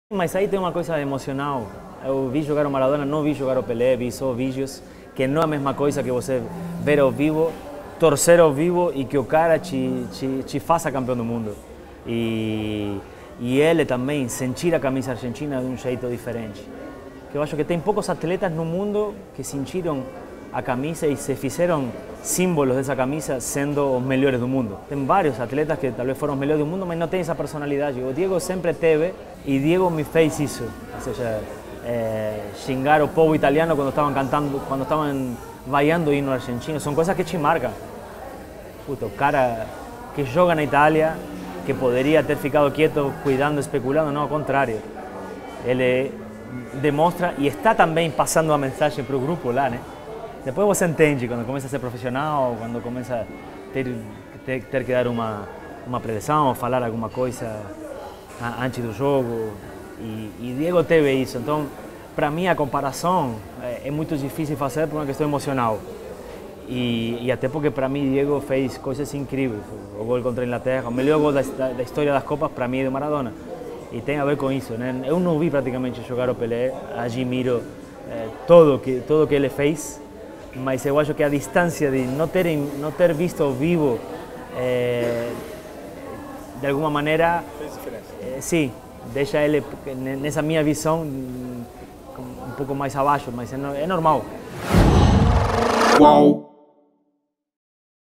Entrevista com o jogador Sorín - "Pelé ou Maradona?"
A palavra pode ser estranha, mas a definição é precisa. Sorín é tarado por futebol. Quando a TV a cabo era raridade, planejava a semana pelos jogos que seriam transmitidos. A grande paixão é a camisa da Argentina. O cara é tão apegado que não gostava nem mesmo de trocar de uniforme no final dos jogos pela seleção...
Mas quem controla o destino? Quis a vida que Sorín se apaixonasse pelo Cruzeiro e depois pelo Brasil. E é aqui, e não a Argentina, o local escolhido para viver depois da aposentadoria. No país, criou um dos programas esportivos mais apreciados pelos jogadores de futebol verde-amarelos, o Resenha ESPN. O sucesso na TV só aumenta os laços com o Brasil.
A entrevista com o UOL Esporte tem um sotaque forte. Mas é inegável que, lá dentro do peito do ex-lateral, a cumbia se misturou com o samba.